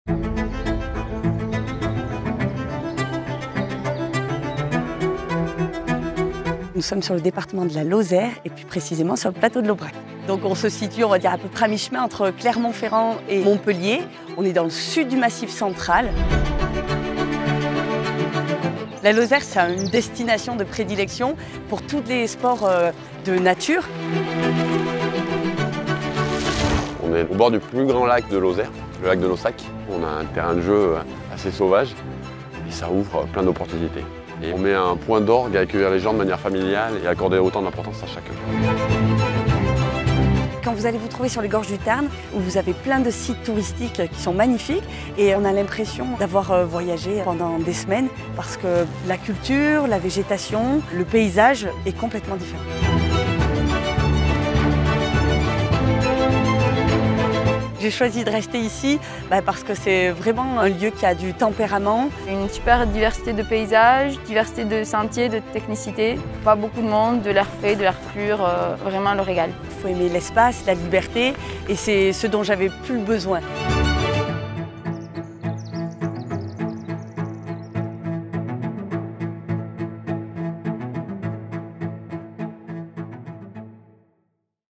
Lozère : L'échappée plaisirs
Choisissez la Lozère pour vos prochaines vacances.

Rejoignez-nous pour une aventure inoubliable au cœur des grands espaces, où la nature préservée vous offre le meilleur terrain de jeu pour vos activités préférées.

Amateur de randonnée, de VTT, de paddle ou de trail, la Lozère vous attend avec ses paysages époustouflants et son patrimoine exceptionnel.

Ne manquez pas cette occasion unique de vous évader et de vous ressourcer